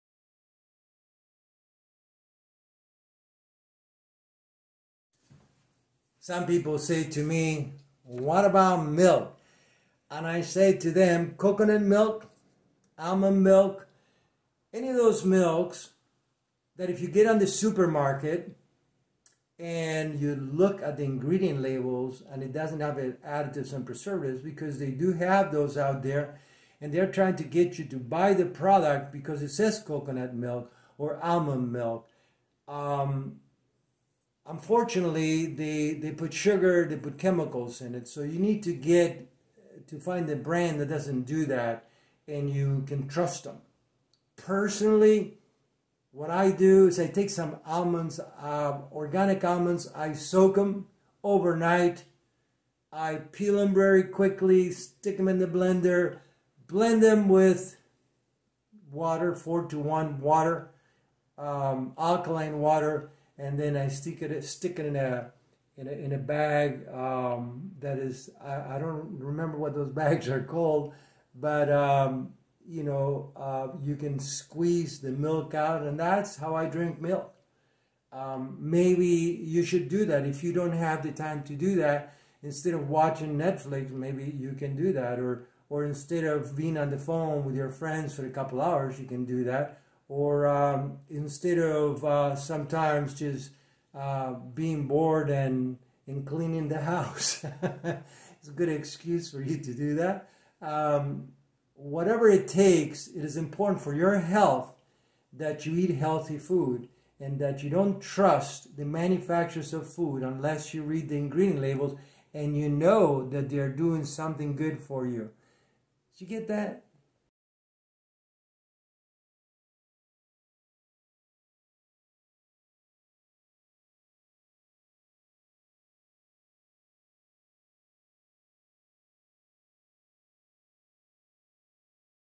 22B - What About Milk?
Milk carries with it any hormone or chemical that the cow has been injected with.  Additionally a great number of people are lactose intolerant, so milk from the cow is not the best choice.  There’re other milks that can replace cow milk, like almond milk, oat-groat milk, coconut milk, etc.  I make my own milk from those products.  If you intend to buy such milks at your local supermarket, make sure you read the ingredient labels.  Manufacturers have a tendency to put excessive sugar and sprinkle some chemicals on such products.